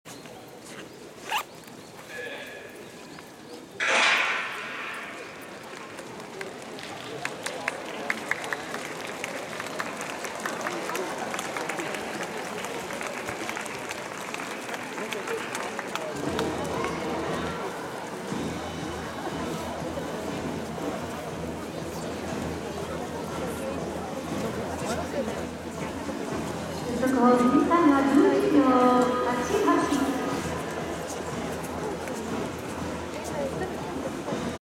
【第57回織田記念陸上】 セイコーチャレンジ 女子100mタイムレース3組
タイムレース3組 (3組 0着+8) (+1.2m/s)

1着  工藤 聖莉奈  11.87  RT 0.168s  “ q ”